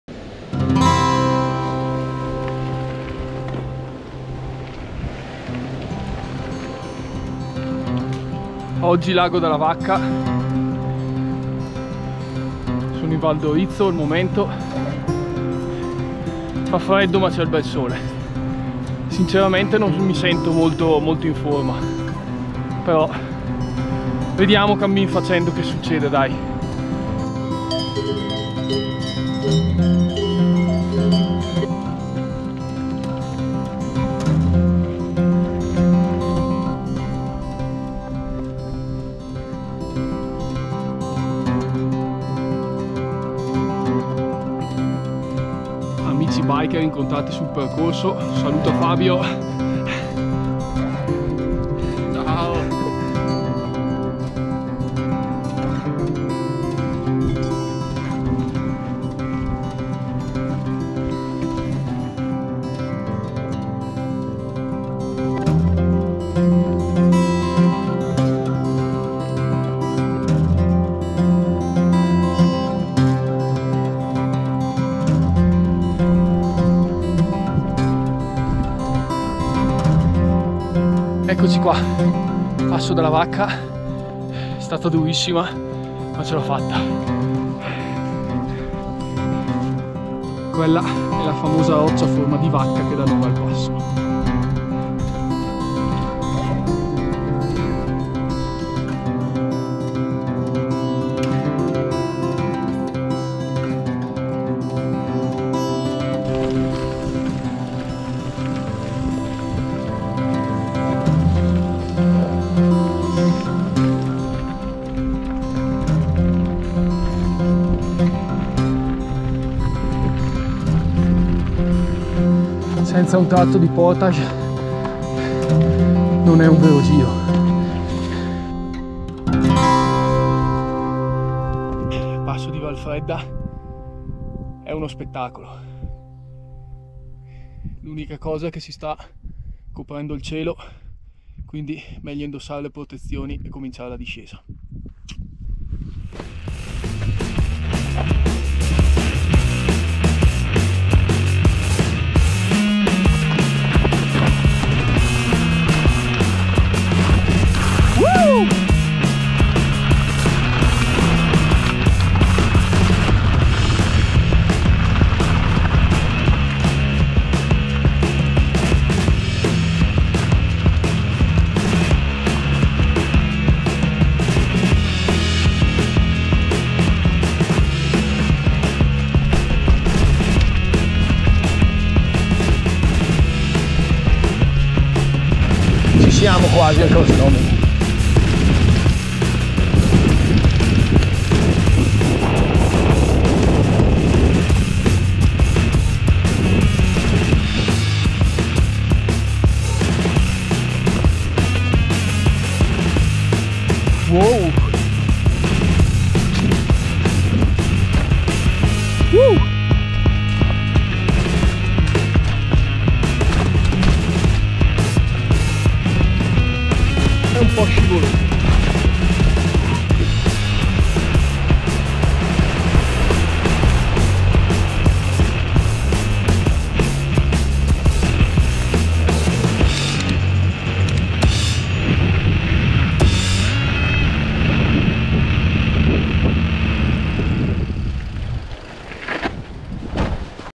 ALE MTB - Lago della Vacca
ALE MTB DAY - un tour in solitaria per un itinerario davvero mitico